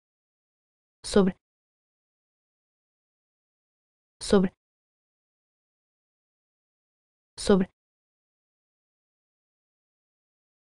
How to say 'on' in Portuguese
In this video you will learn how to say 'on' in Portuguese - sobre. The Learn Portuguese channel helps you improve your language skills and has all the words you need. Listen, learn, and speak Portuguese.